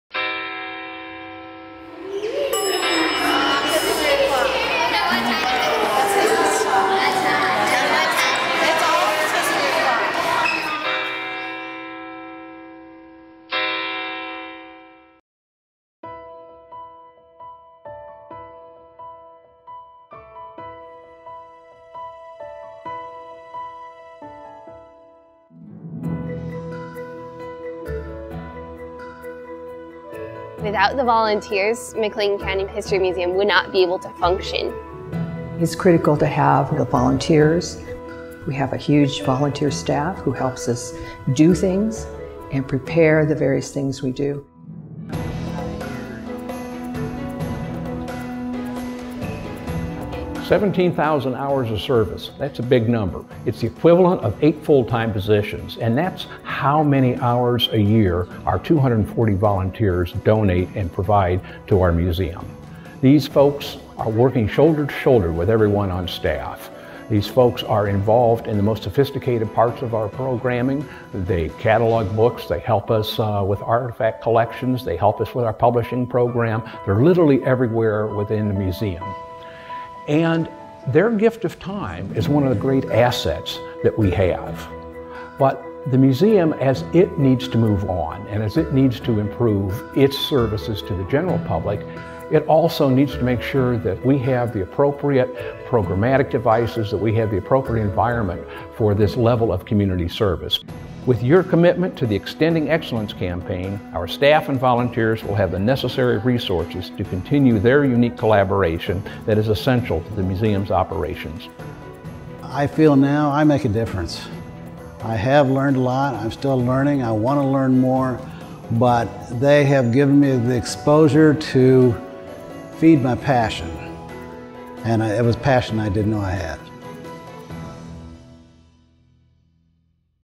Extending Excellence: The Volunteer Experience at the McLean County Museum of History
Board members and community leaders have collaborated to launch Extending Excellence: The Campaign for the Museum of History.  This campaign will raise $3 million to install a new permanent exhibit, embrace new technology, expand the capacity of the Education department, and upgrade landscaping.